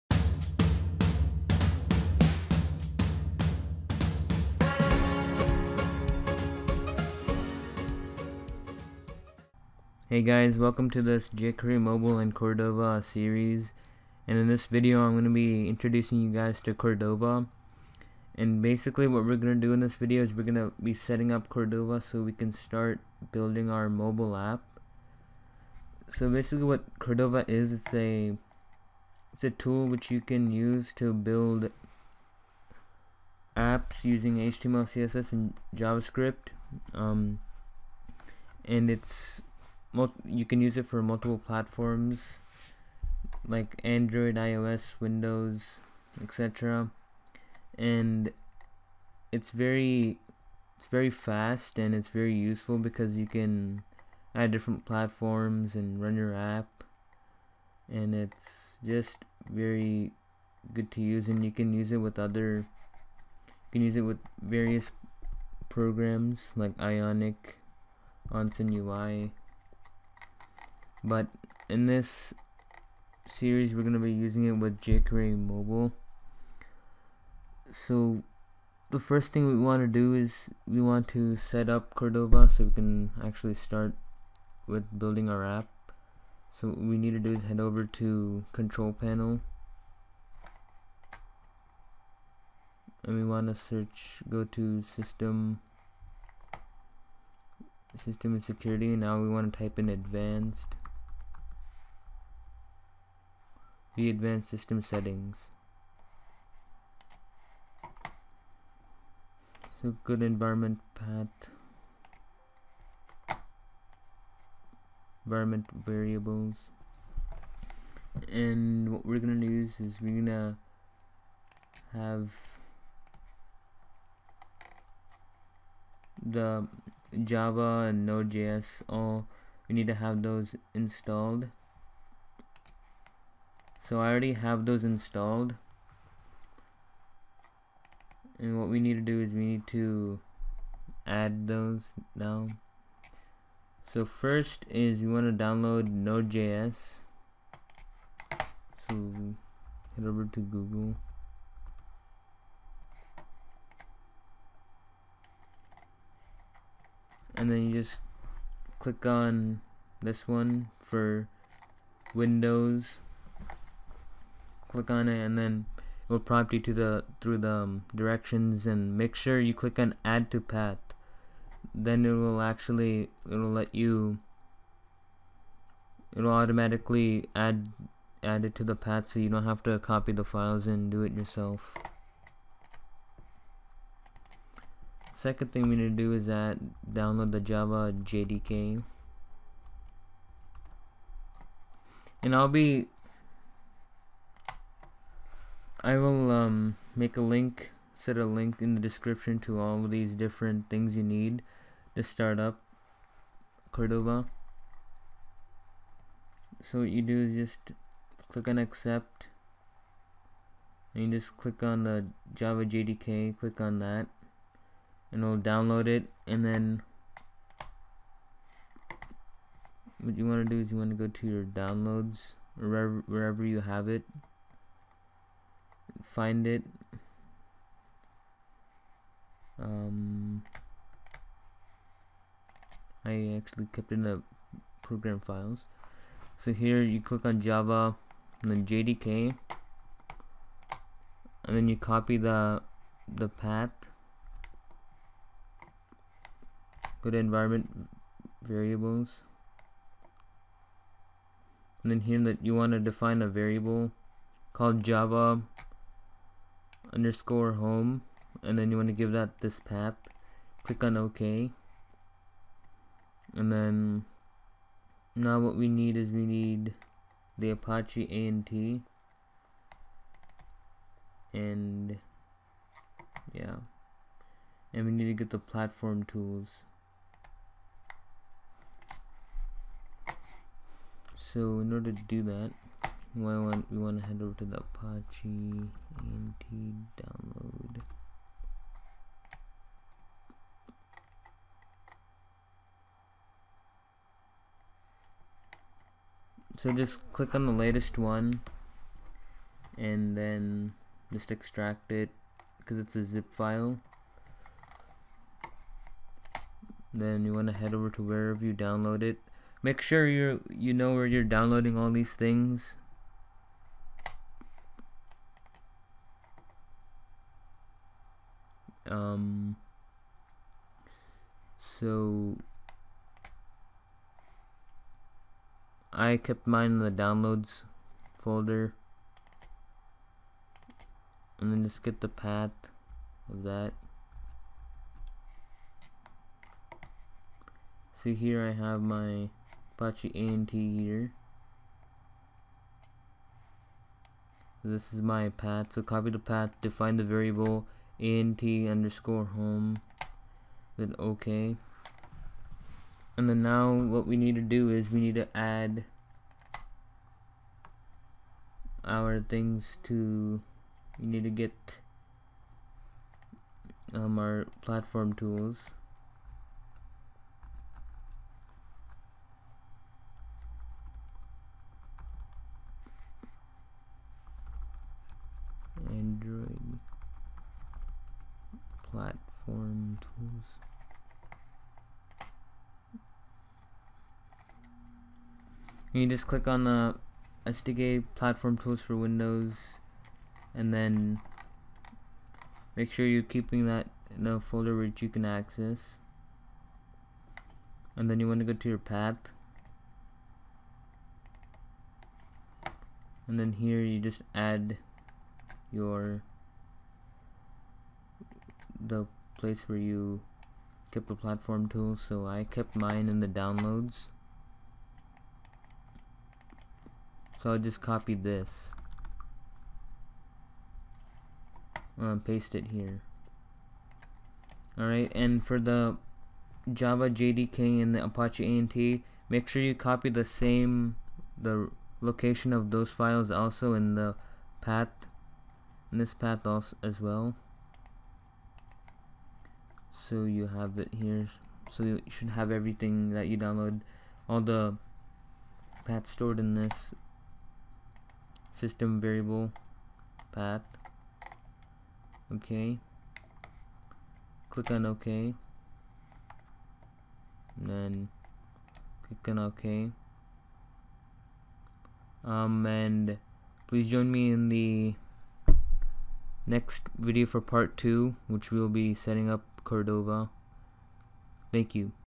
Setting up/Installing Cordova - Part 1
This is a very easy tutorial on how to setup Cordova. Make sure you download the Java JDK, Nodejs, Apache Ant, and Android Platform Tools. If you have any questions, please ask below! This is my first video on Youtube!